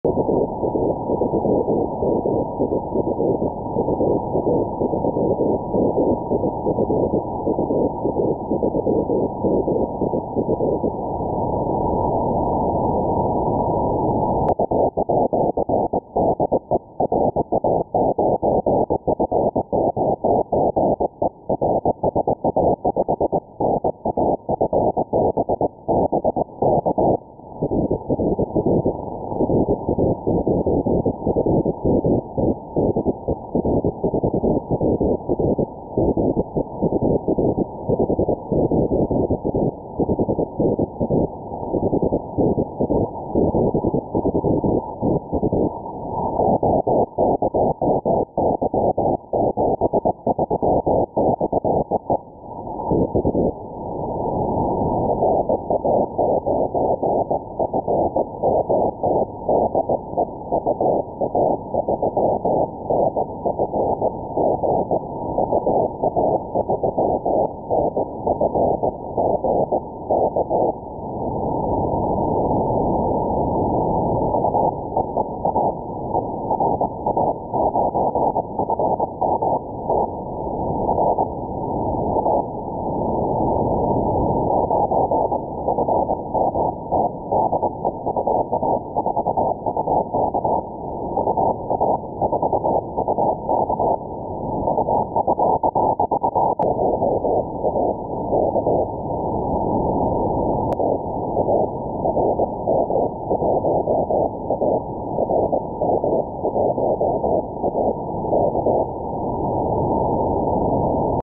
Aurora on VHF 144 MHz 7 Nov-2022
Antenna: 4x9 elem YAGI
BPF, LNA, SDR Airspy HF+ Discovery